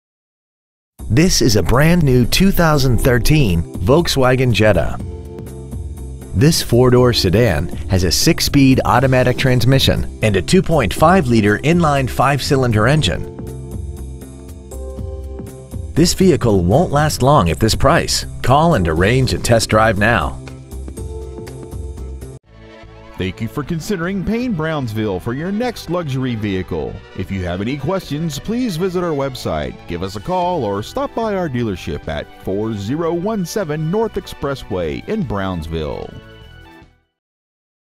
2013 Volkswagen Jetta Harlingen TX
We have been honored to serve the Brownsville tx area , we promise that your experience at our dealership will exceed your expectations !

 Year : 2013
 Make : Volkswagen
 Model : Jetta
 Engine : 2.5L 5 cyls
 Trans . : Automatic 6-Speed
 Exterior : Black Uni
 Miles : 10
 Interior : Titan Black w/Titan Black V-Tex Leatherette Seat T
 Stock : M351217

 4017 N. Expressway